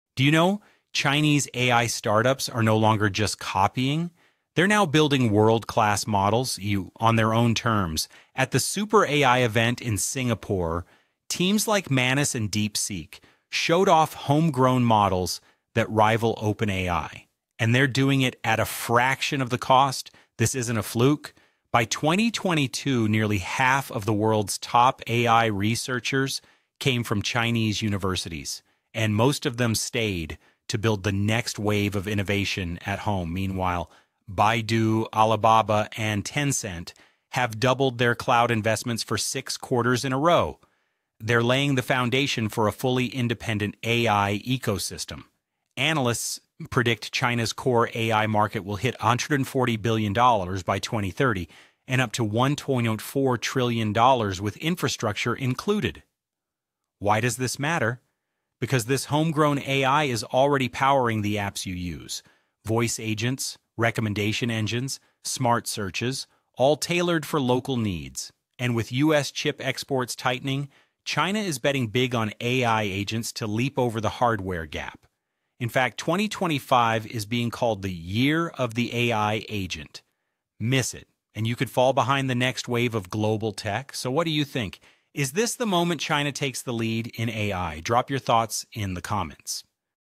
No More Copycats: China’s AI Startups Are Going Global
Chinese AI startups are no longer just copying — they’re building powerful, homegrown models that rival OpenAI at a fraction of the cost.

Backed by top talent from Tsinghua and Peking University, and fueled by huge investments from Baidu, Alibaba, and Tencent, China’s AI ecosystem is rapidly scaling.

Analysts expect the core AI market to reach $140B by 2030 — and $1.4T when infrastructure is included.

And with U.S. export controls tightening, China’s betting on AI agents, not chips, to leapfrog the global AI race.

💬 Do you think this homegrown revolution will change the way you search, chat, or work with AI?